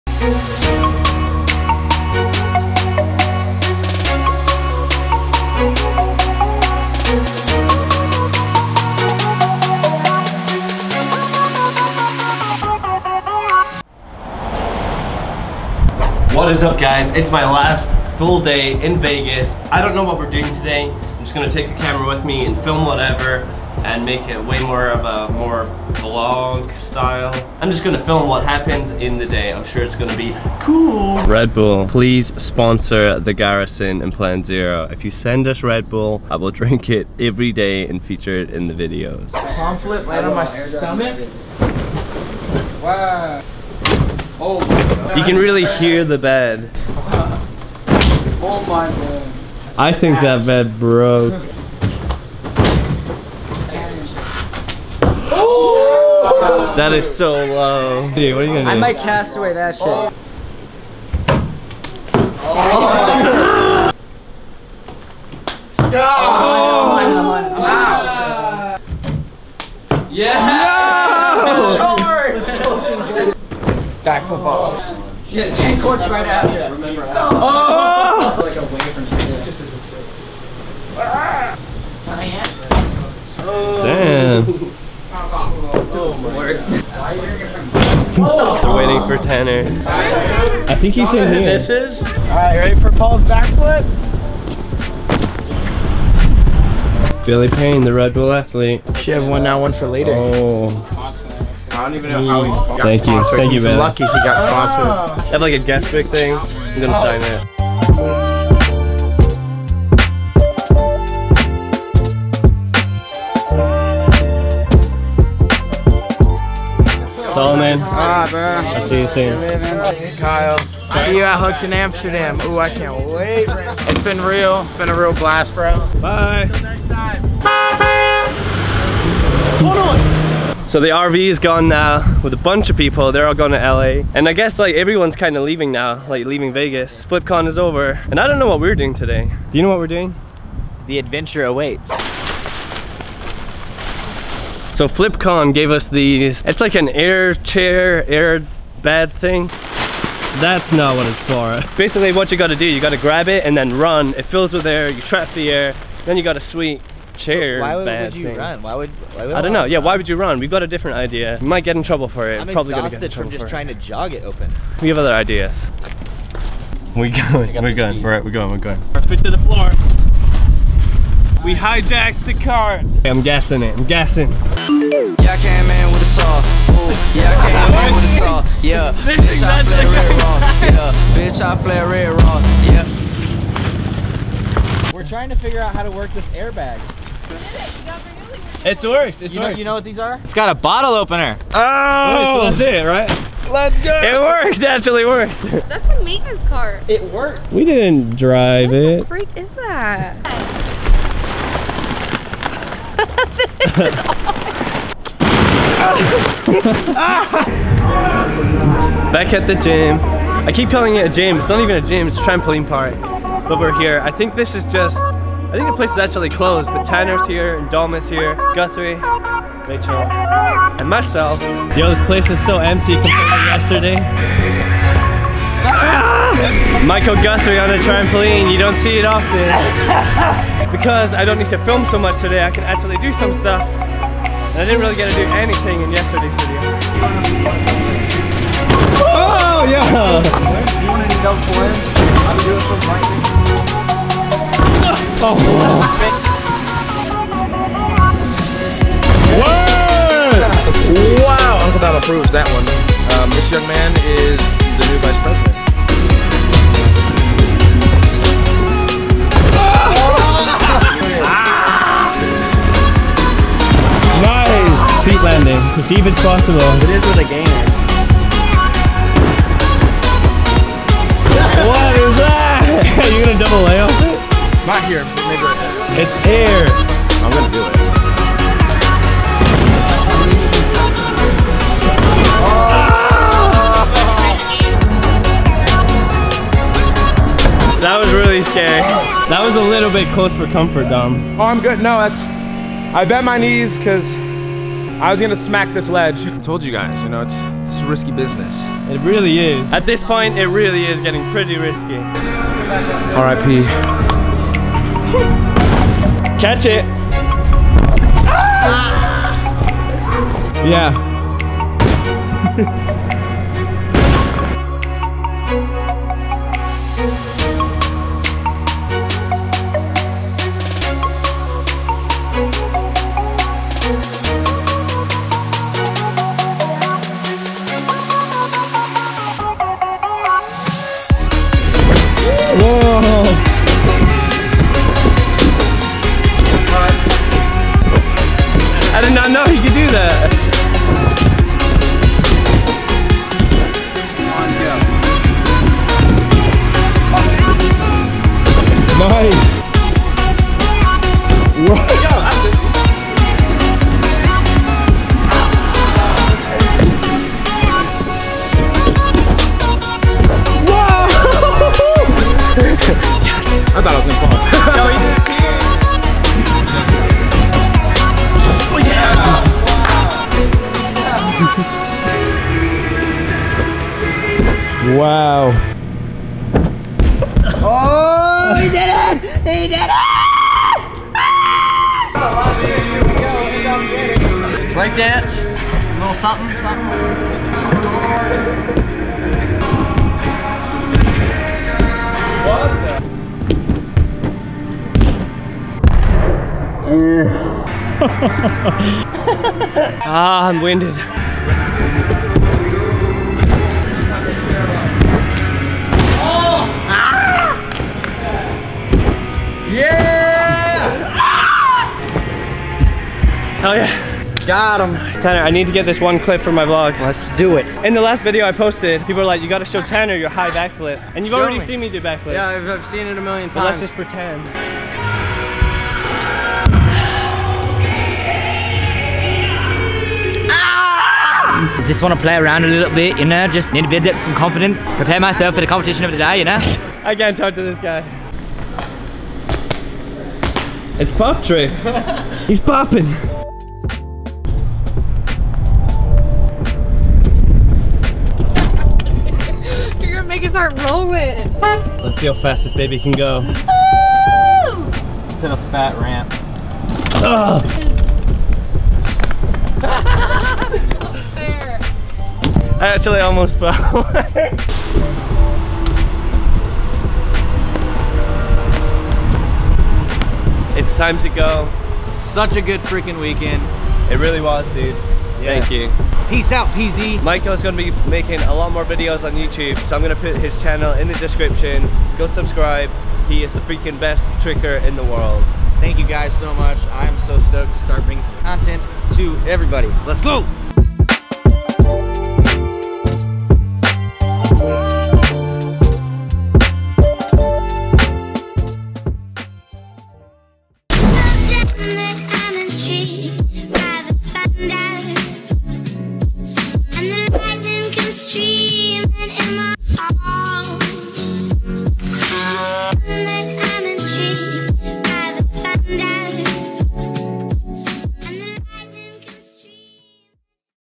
PRIVATE TRAMPOLINE PARK WITH A CELEBRITY!
My last full day in Vegas. Some flips in the hotel, then a private trampoline session just to ourselves. With Tanner Braungardt, Michael Guthrie, Domitrick, Quentin Mcconathy

MY MAIN CAMERA
VLOG CAMERA
MAIN WIDE ANGLE LENS
GOOD ZOOM LENS
CHEAP BUT GOOD LENS
LENS ADAPTOR

kyleslyf